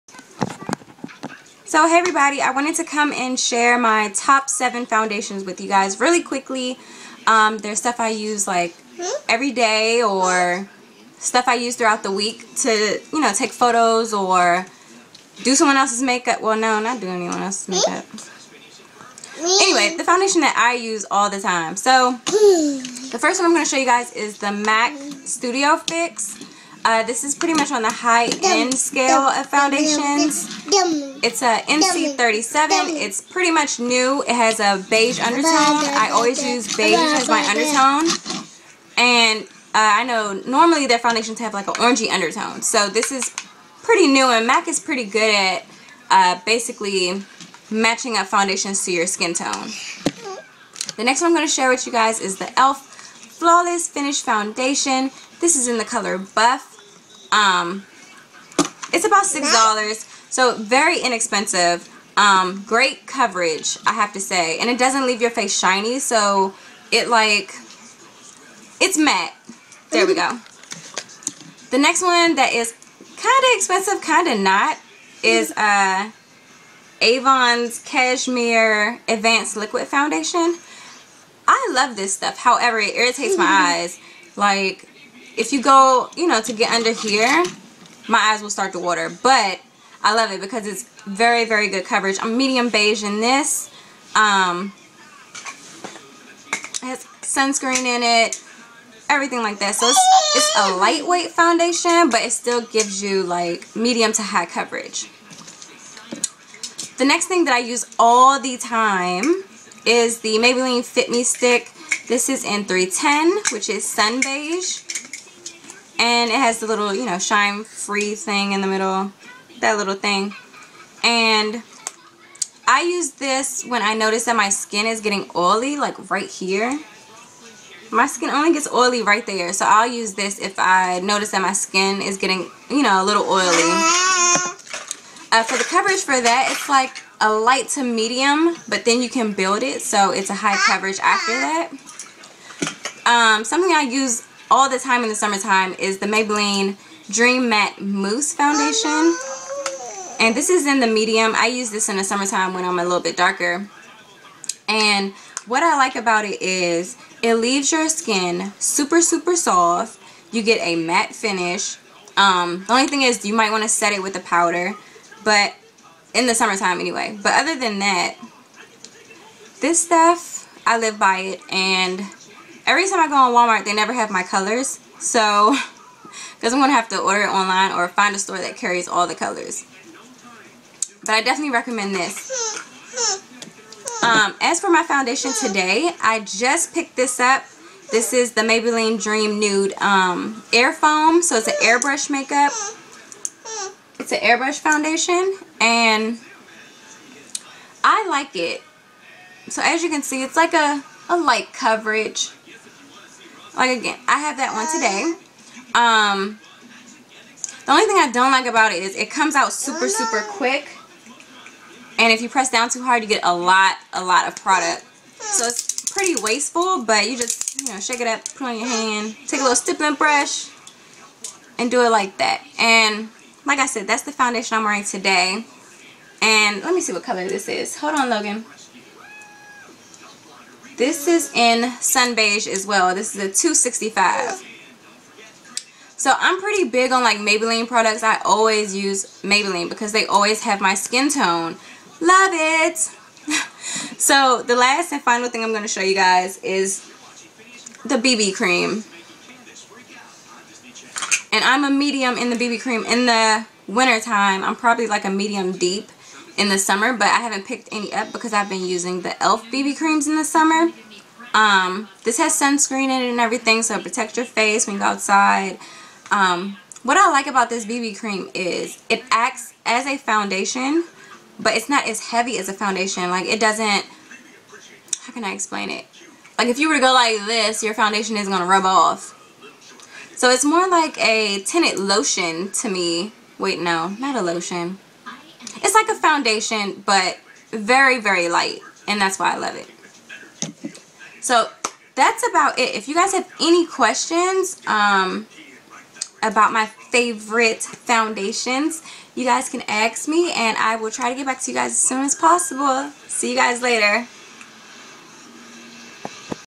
My top 7 foundations
Mac studio fix nc 37
Maybelline bb cream in medium
Maybelline dream nude airfoam in sun beige
Maybelline fit me stick in sun beige
Avon cashmere advanced liquid foundation in medium beige
Maybelline dream matte mouse medium beige
Elf flawless finish foundation in buff